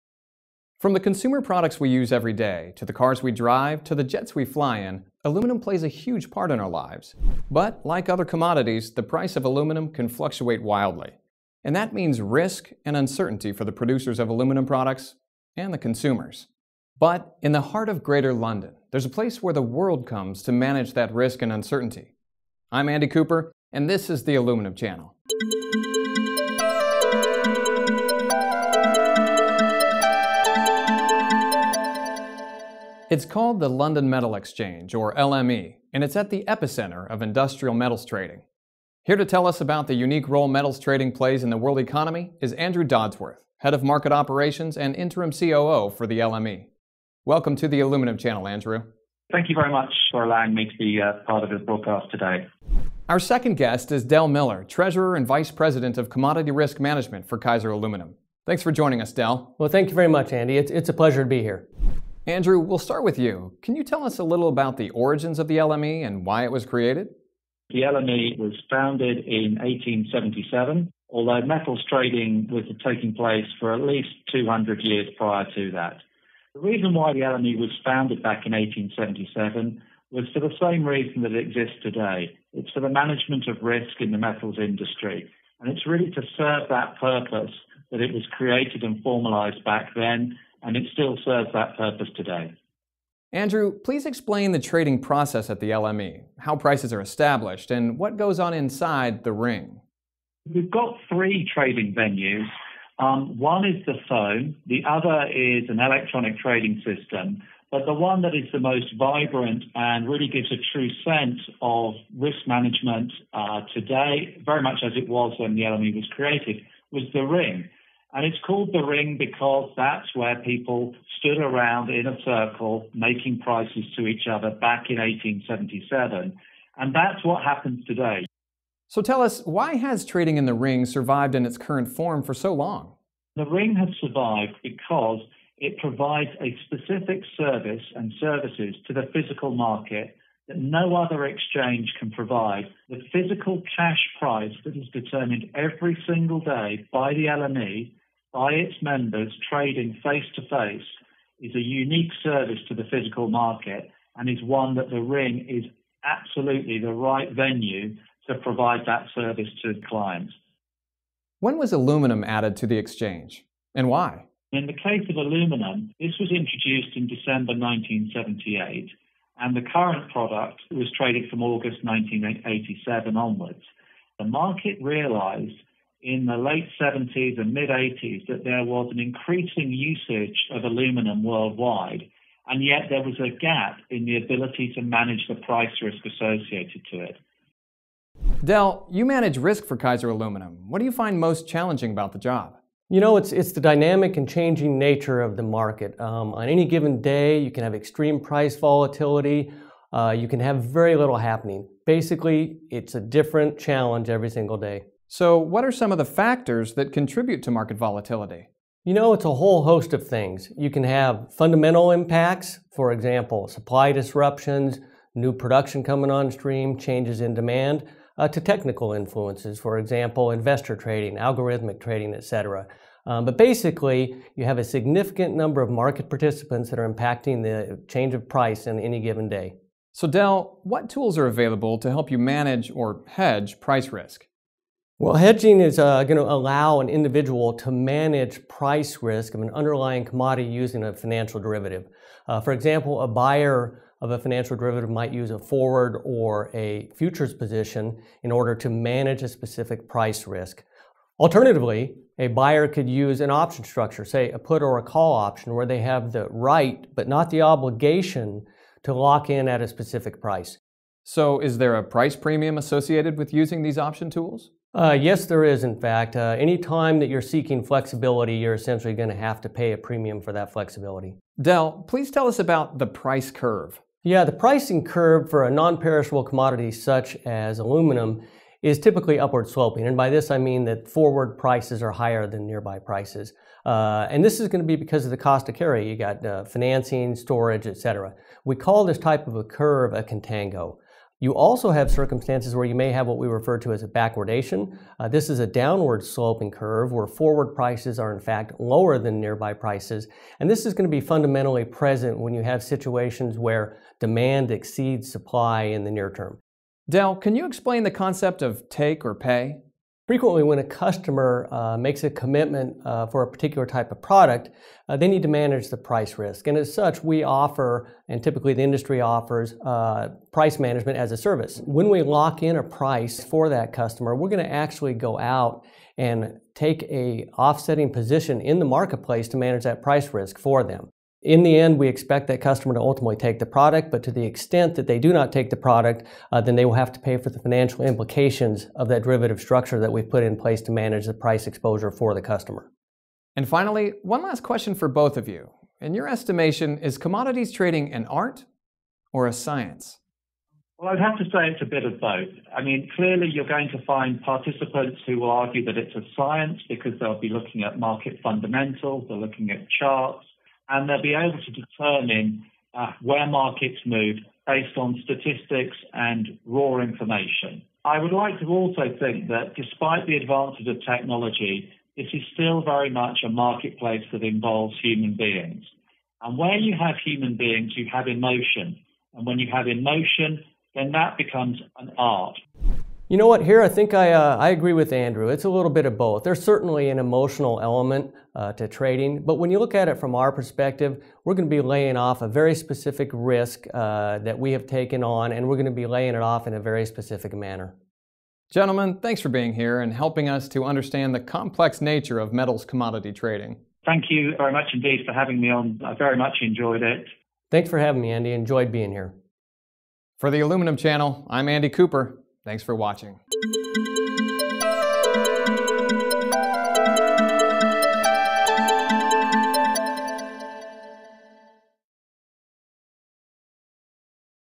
The London Metal Exchange and Aluminum Price Risk Management
The London Metal Exchange, or the LME, is 140 years old this year but buyers and sellers of metals met in a “ring” 200 years earlier trading metals almost the same way they do today.  In this episode, Andrew Dodsworth, Head of Market Operations and Interim COO for the LME and Del Miller, Treasurer and Vice President of Commodity Risk Management for Kaiser Aluminum, discuss the origins of metals trading, specifically aluminum, and the tools producers and consumers can use to manage the risks associated with price volatility.